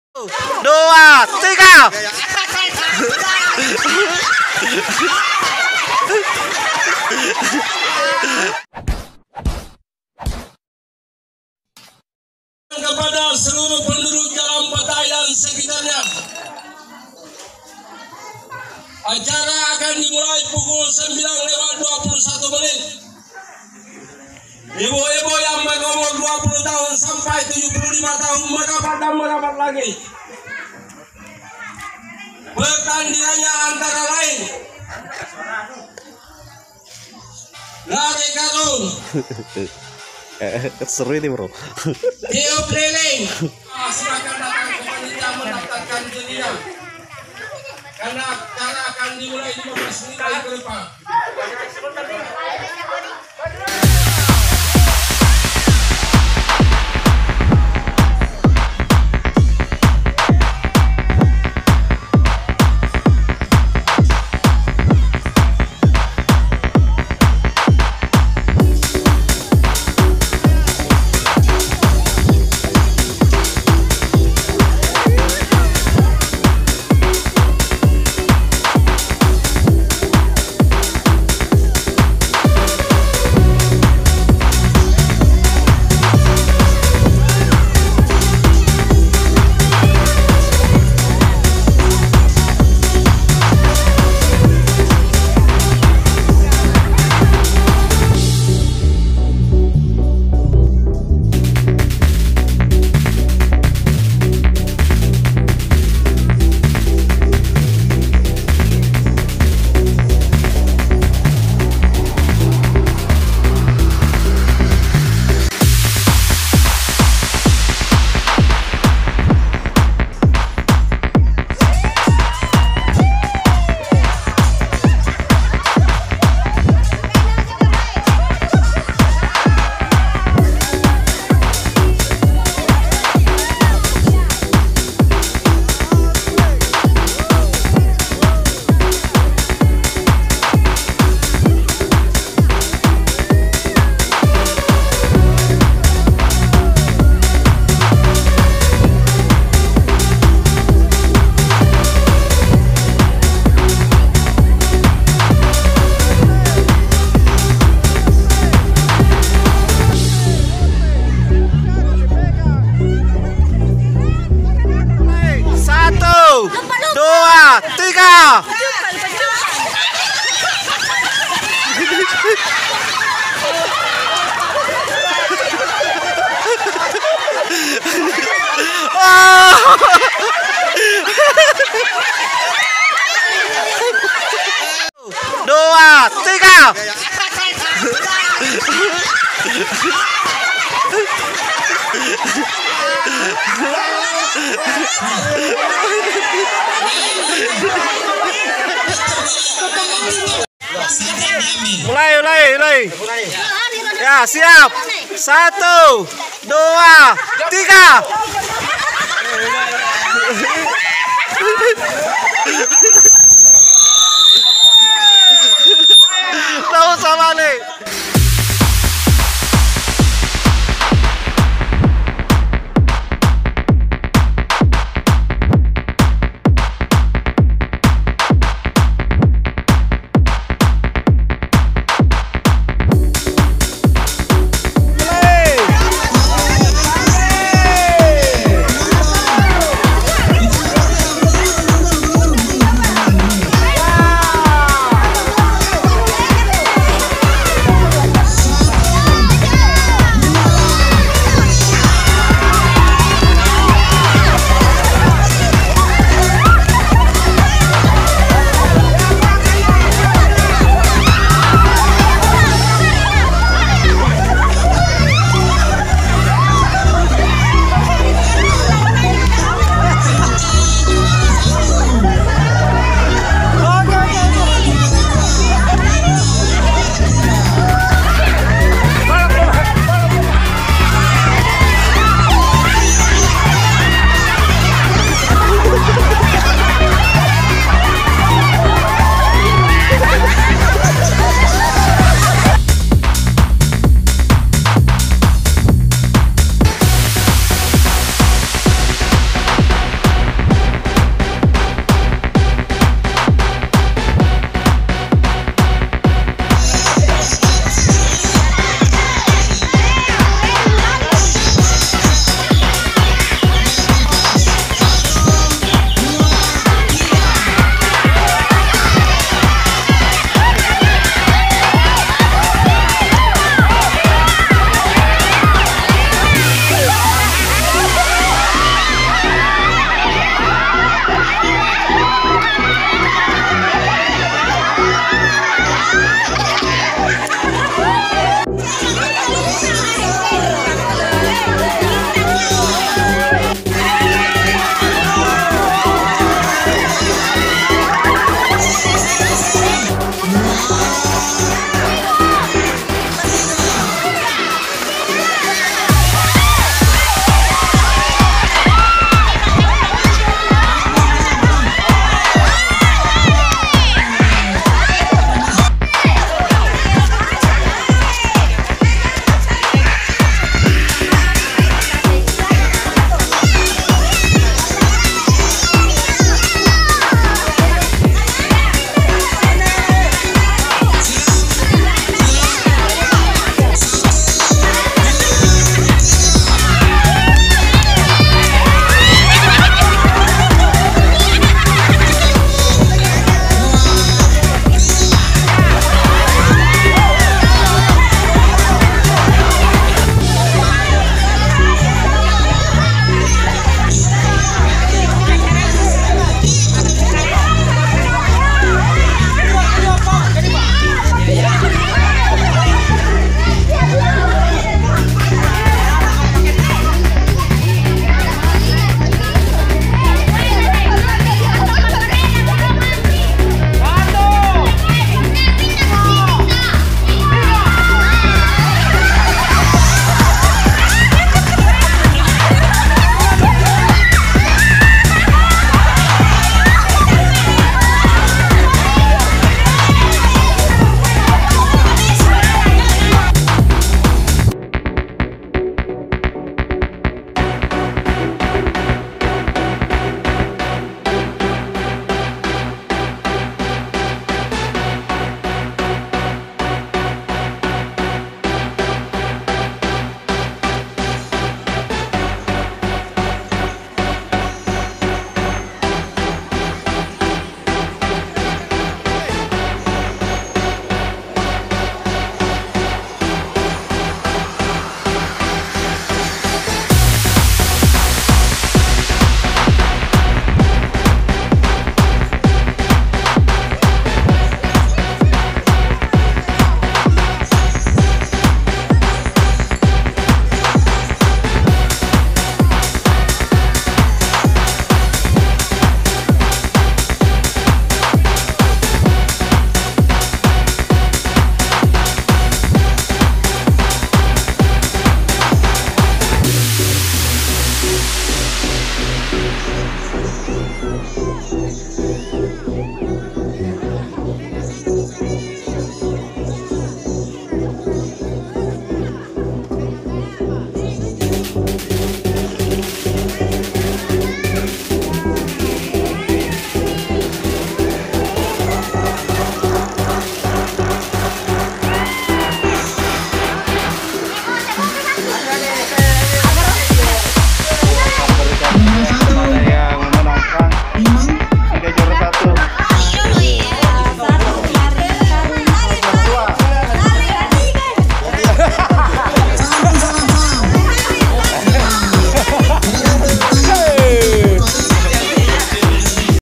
Video Viral Lucu Warga Merayakan Hari Kemerdekaan Republik indonesia
video Viral Lucu Warga Merayakan Hari Kemerdekaan Republik indonesia yang sangat sederhana tapi mengocok perut